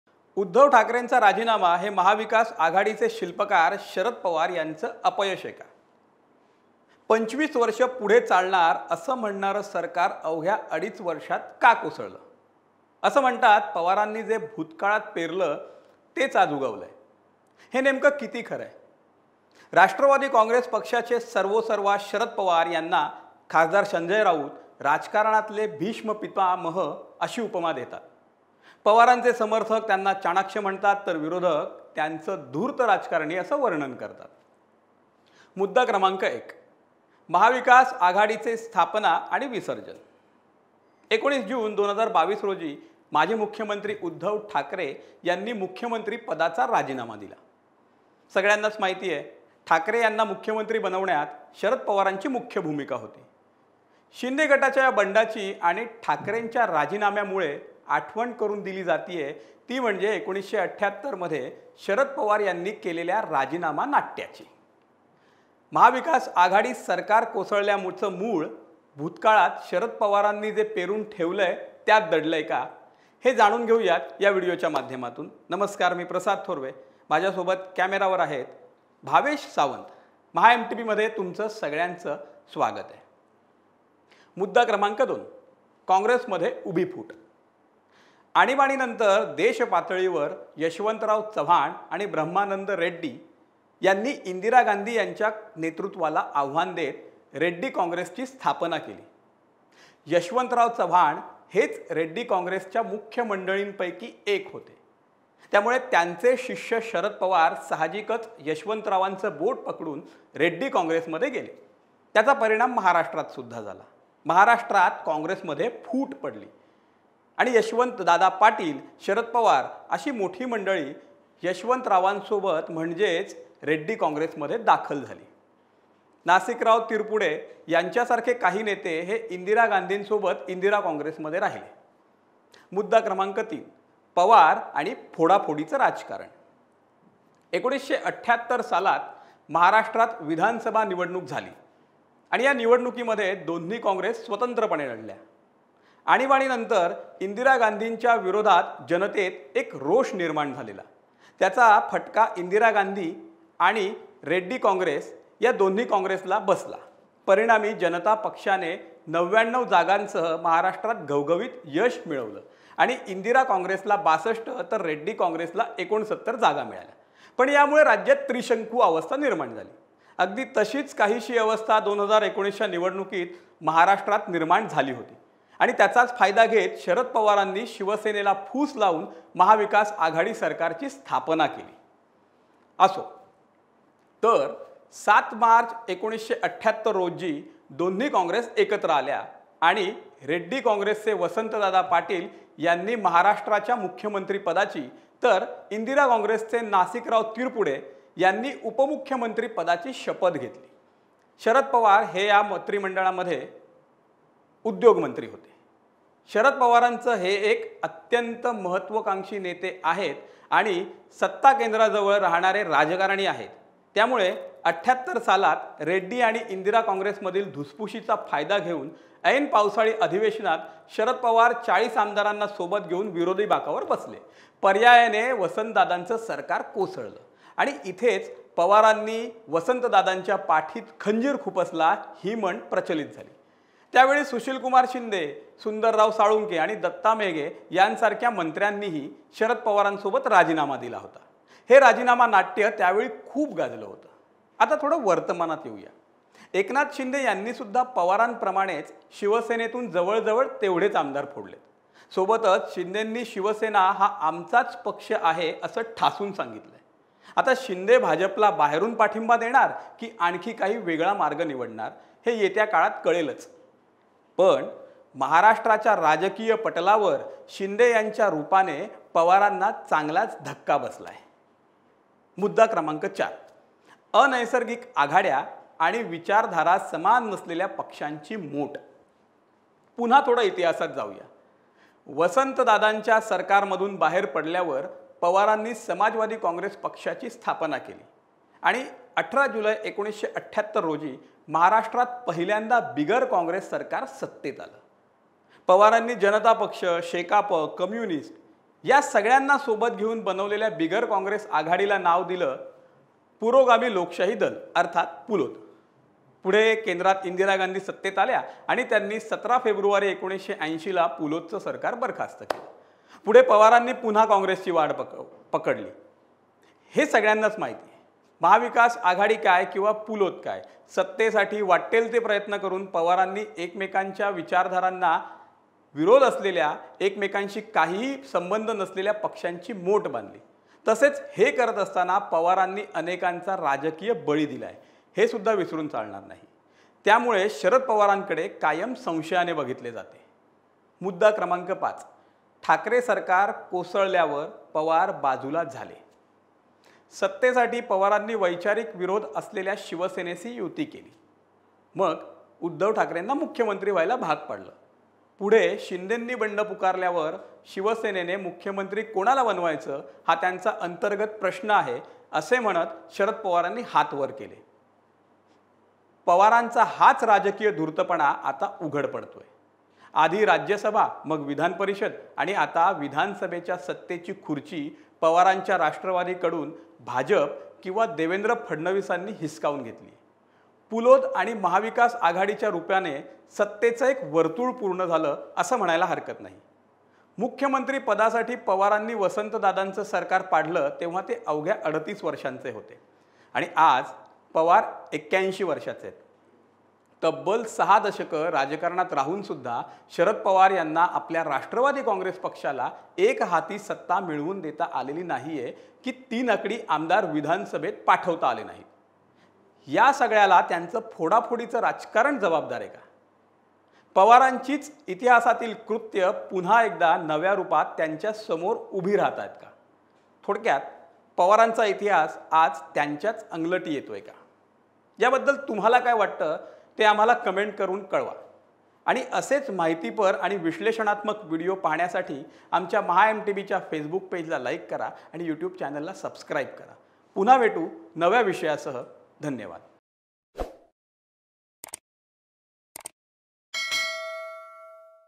पवारांनी जे पेरलं तेच आज उगवलं!
तब्बल ६ दशक राजकारणात राहून आणि प्रचंड क्षमता असून शरद पवार यांना आपल्या राष्ट्रवादी काँग्रेसला एकहाती सत्ता मिळवून देता आली नाही की ३ आकडी संख्या असलेले आमदार विधानभवनात पटत आले नाही. यासगळ्याला त्यांचं राजकारण जबाबदार आहे का? इतिहास त्यांच्या अंगलटीला येतोय का? जाणून घ्या या व्हिडिओतून.